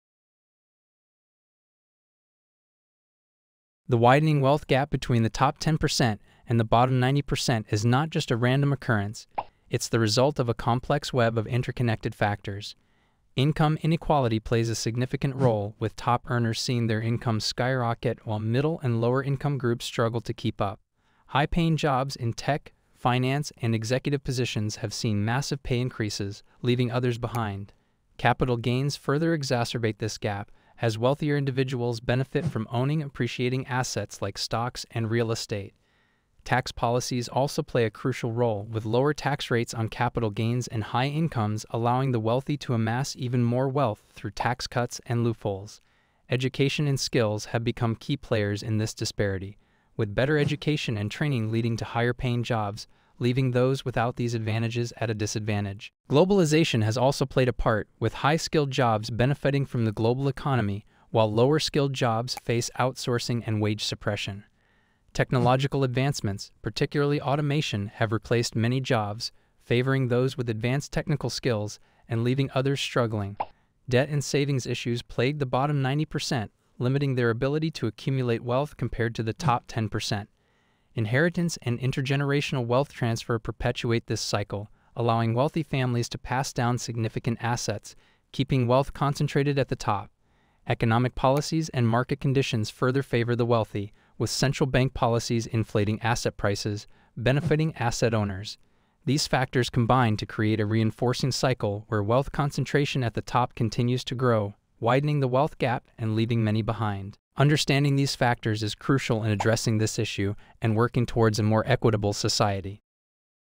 Income Inequality - The Growing Wealth Gap Disparity
In this short video, you'll learn the reasons for income inequality and the growing wealth gap disparity.